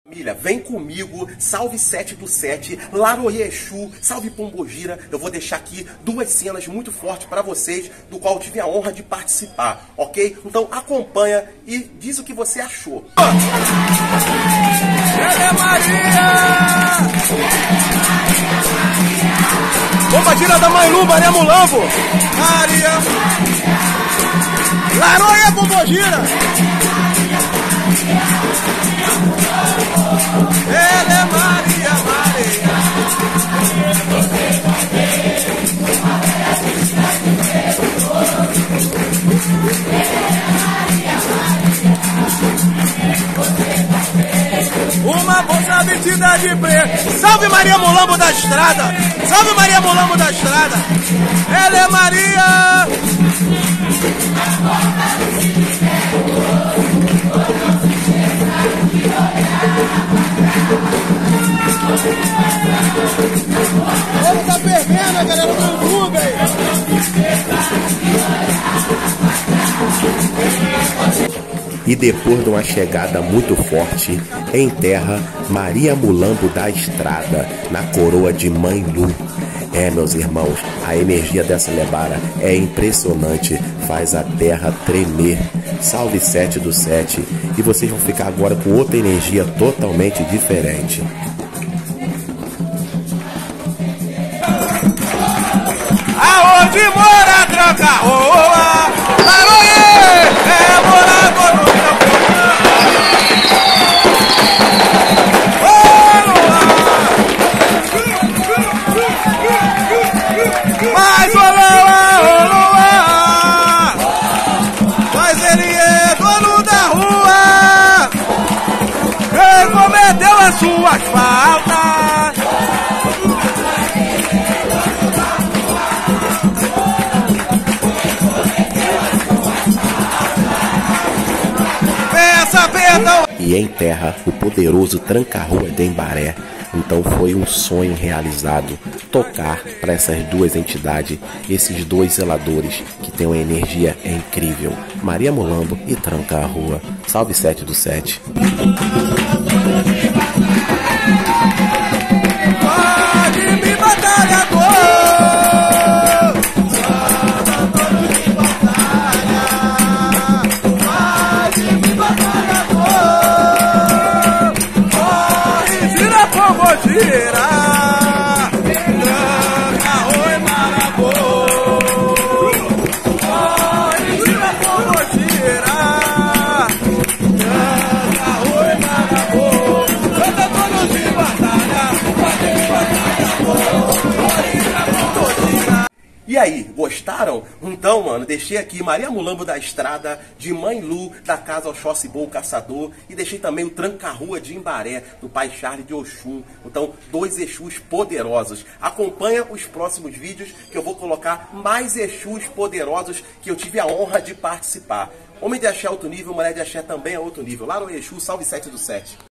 MARIA MULAMBO & TRANCA RUA/ Mãe Lu Oxóssi ibo Caçador e Pai Charles de osun/ INCORPORAÇÃO Exú 🔥
QUE GRATIFICANTE 🔥🔱🔥
TIVE O PRAZER DE TOCAR
PRA 2 EXÚS ESSE ANO,QUE AMO DE PAIXÃO.

E DUAS PESSOAS QUE SÃO
DE EXTREMA CONFIANÇA 🙌

MÃE LU E SUA FAMÍLIA

E O PAI CHARLES D' OSUN.

OS DONOS DO MEU CAMINHO

MULAMBO E FALANGE🔱

TRANCA RUA E FALANGE🔱

GRATIDÃO PELA CONFIANÇA
PELO TRABALHO QUE VENHO FAZENDO.

⚠️ VAMOS DEIXAR UM COMENTÁRIO
PRA MÃE LU E PAI CHARLES.

POIS JÁ PENSOU UMA GIRA
COM OS 2 JUNTOS ???

LAROYÊ E SALVE 7/7 EXÚ 🔥